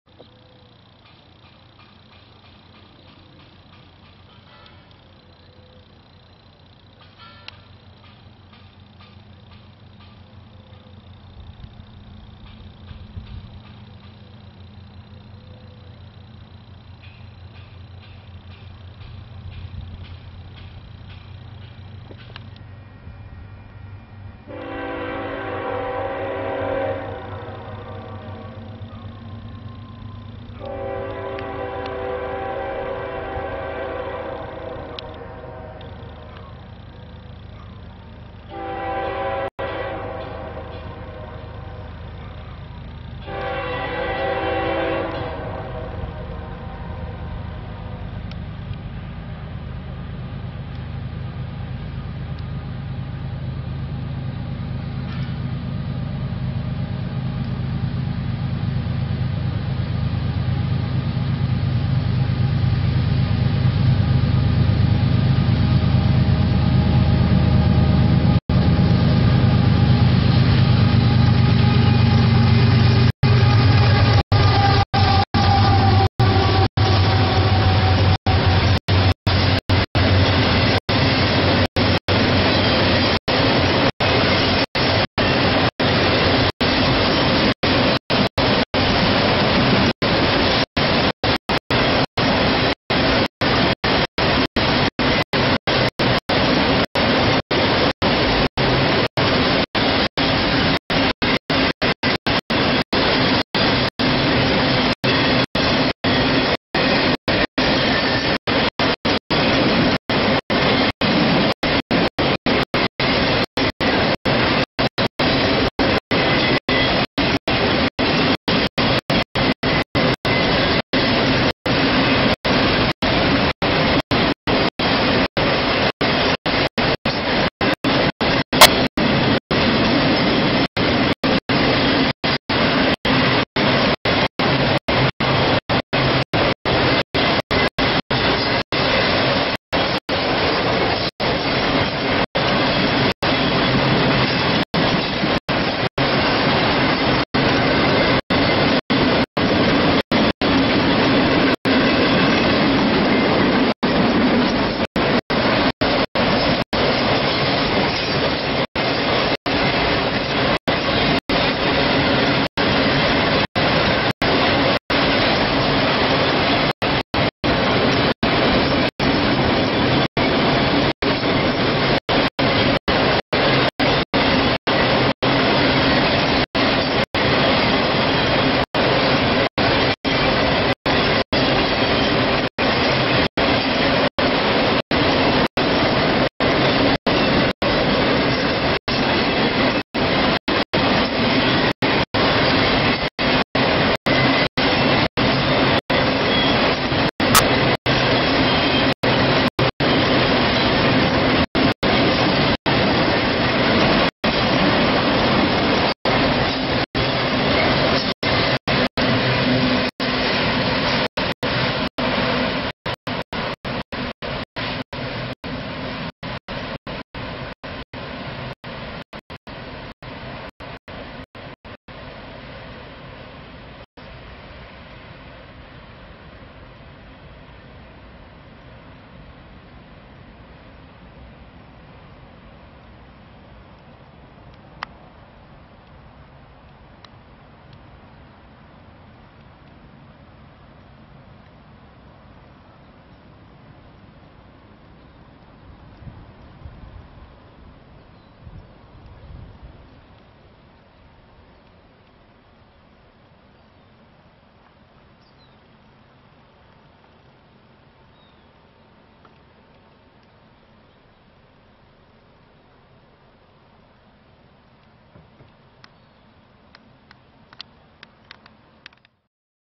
UP 8082 empty autoracks south [HD]
An AMPEPR (UP 8082, 3882, 70 racks) comes through south- central Atascadero CA at 12:15 PM on Mon 13 Jul 2015.